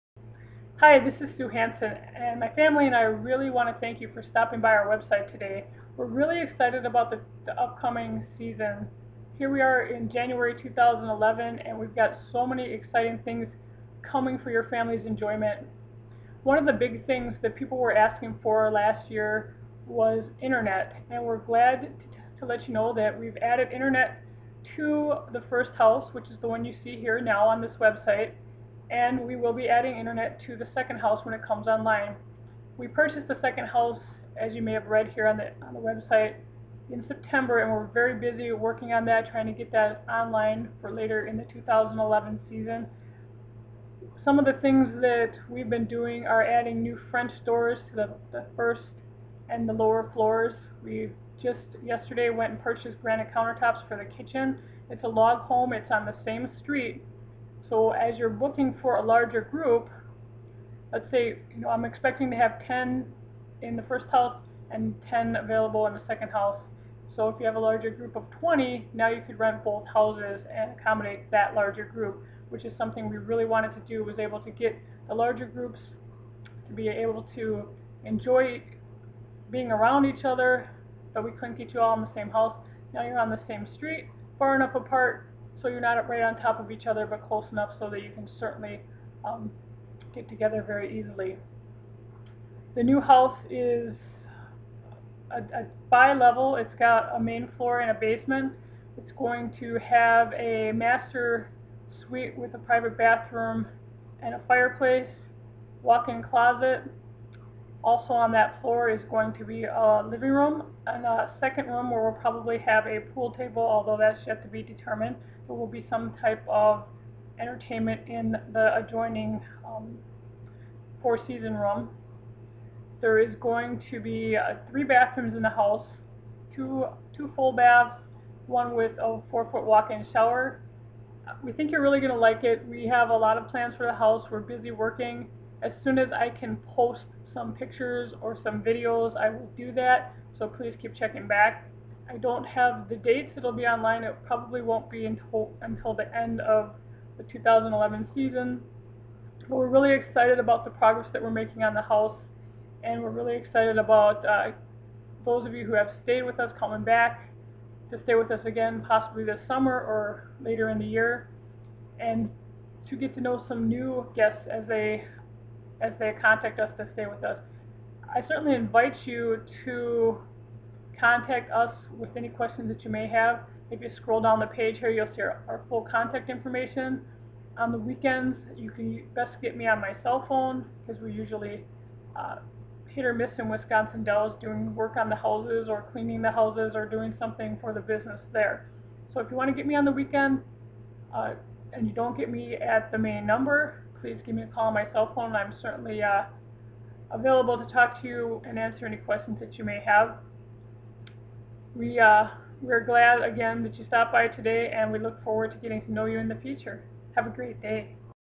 Thank you for visiting our website
Welcome video for our Wisconsin Dells vacation home rental website.